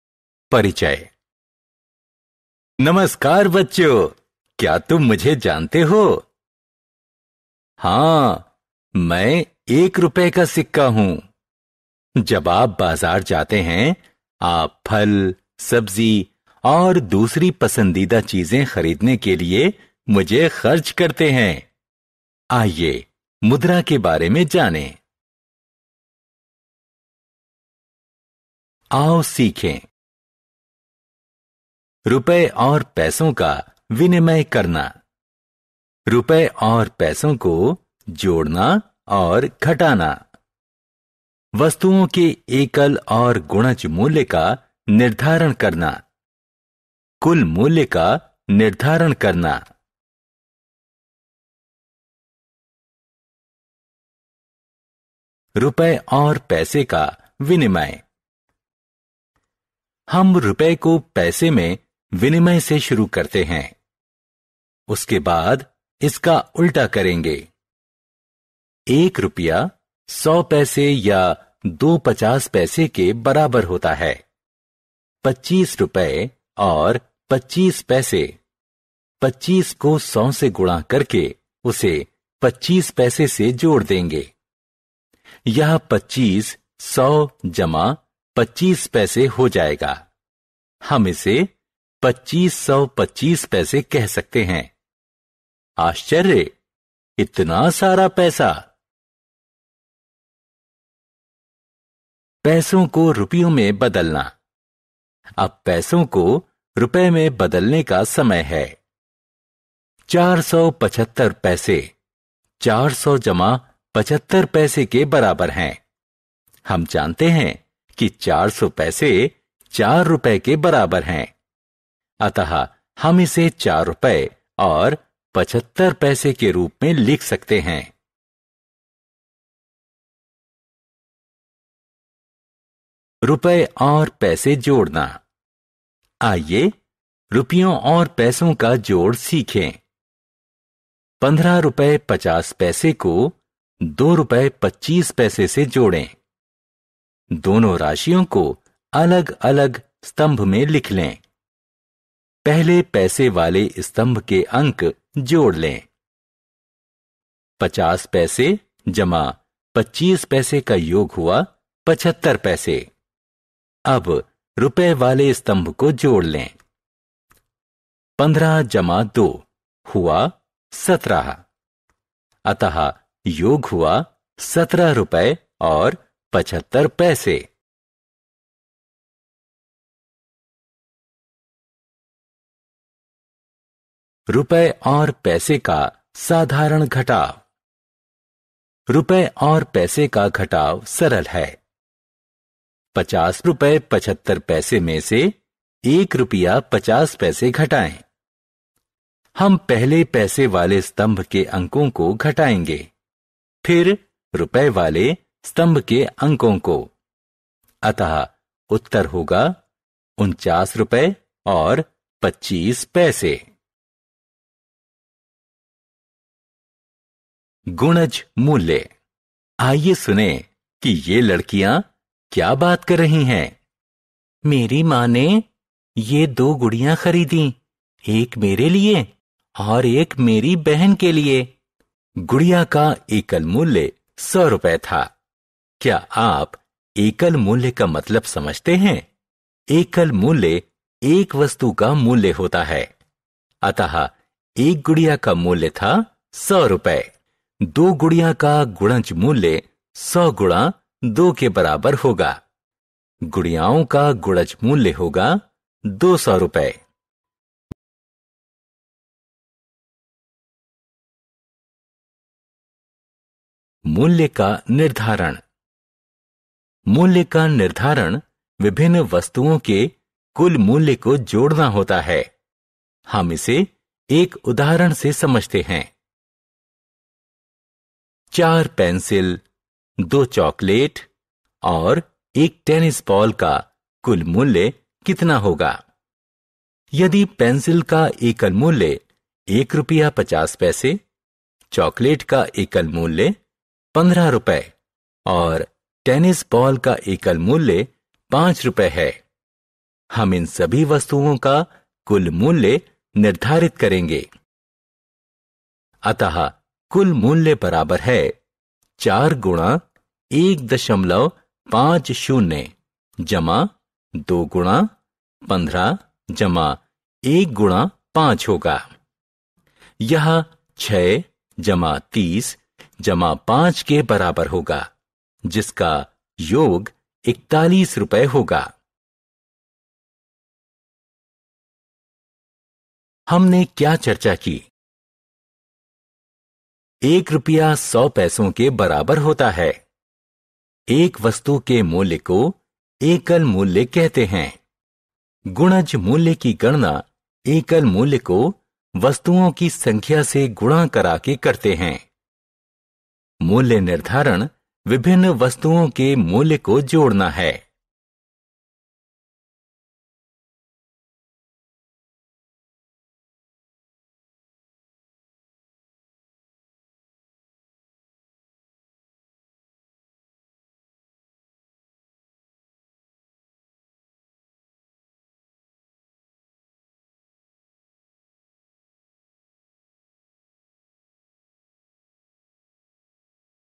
मुद्रा (Money) l कक्षा-4
In this session students will get the information about the Money.

Clear your subject-oriented doubts on a one to one session with your favorite Teacher.
So join the telegram group now,